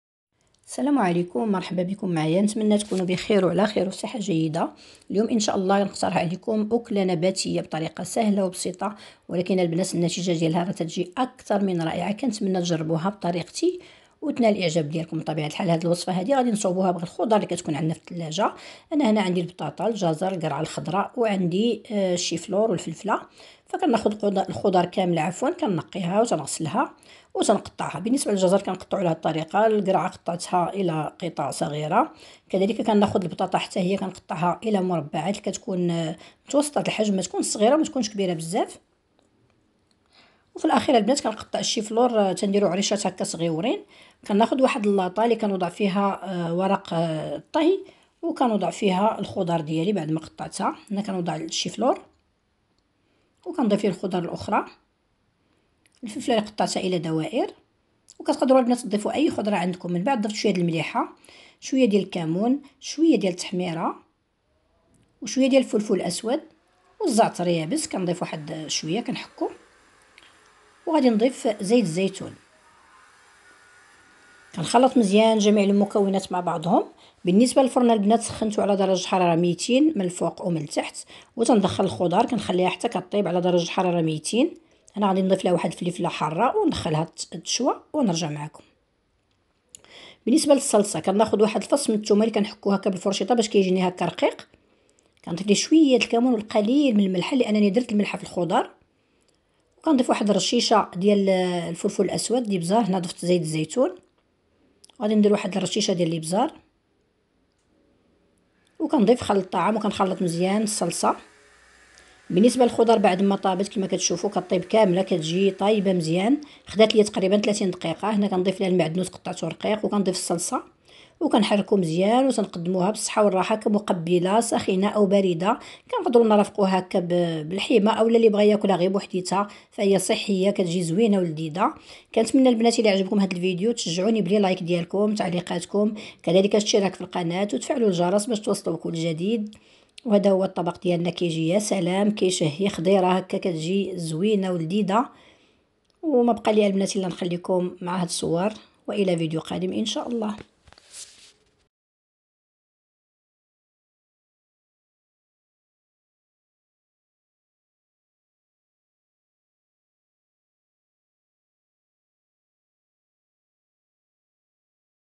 إلآ عندك خضار مشكلة فالثلاجة جربيهوم بهاد الطريقة وردي عليا لخبار 😋😋😋👍
سنية خضار مشوية فالفرن بطريقة واااعرة كتشهي